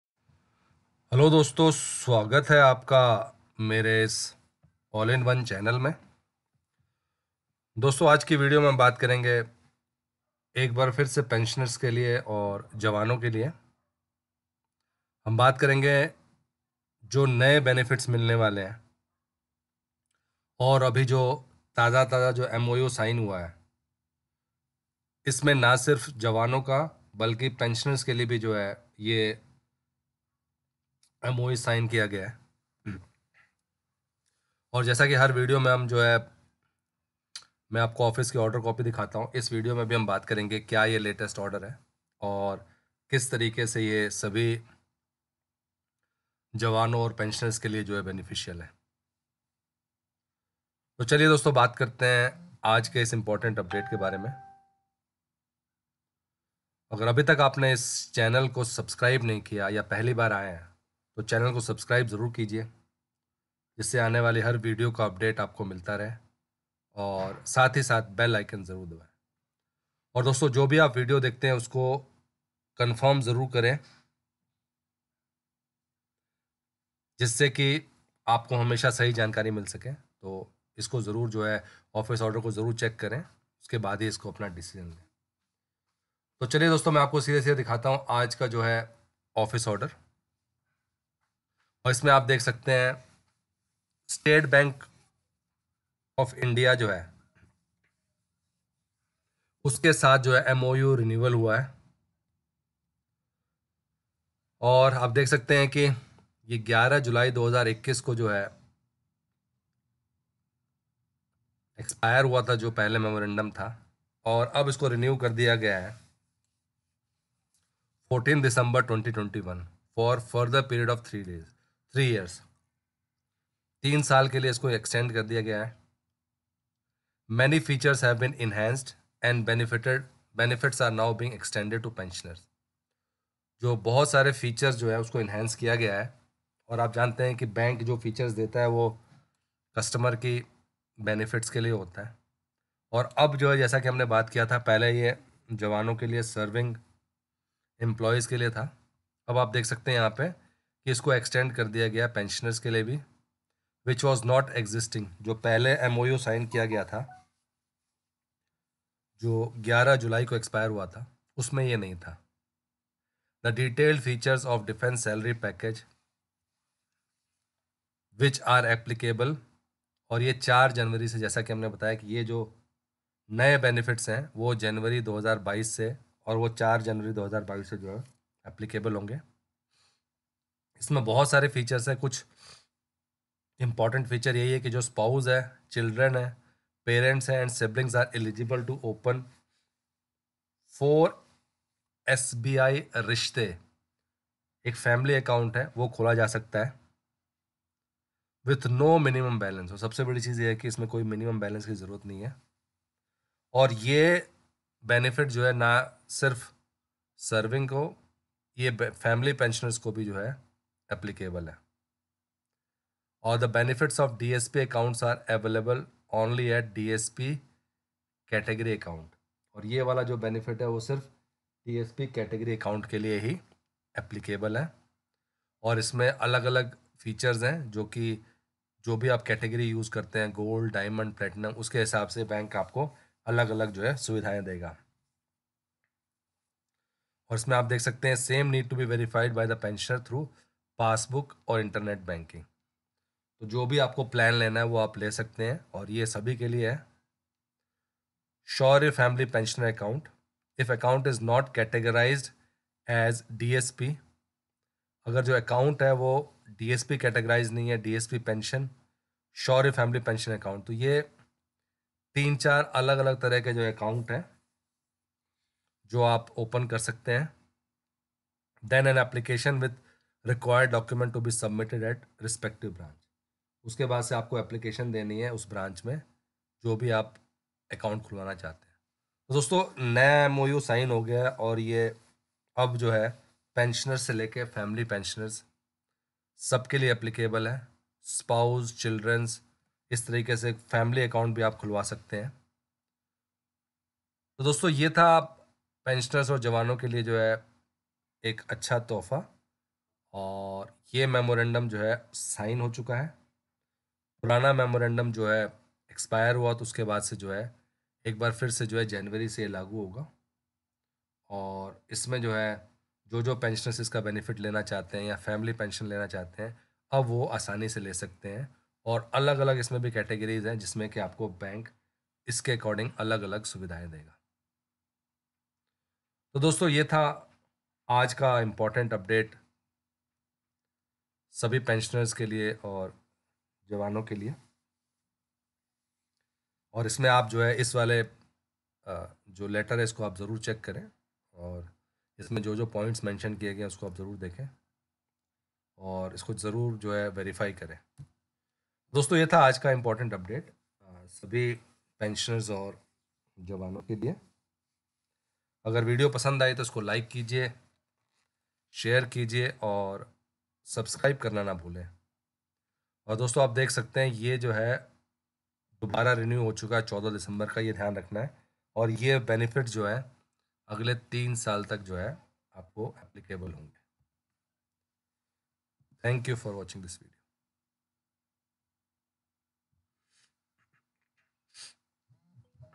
सभी पेंशनर्सऔर जवानोंके लिए सरकार की तरफ से एक और तोहफा !! Pensioners Good News
In this video for all pensioners and jawans I have discussed important updates for all pensioners for the latest order for jawans and pensioners.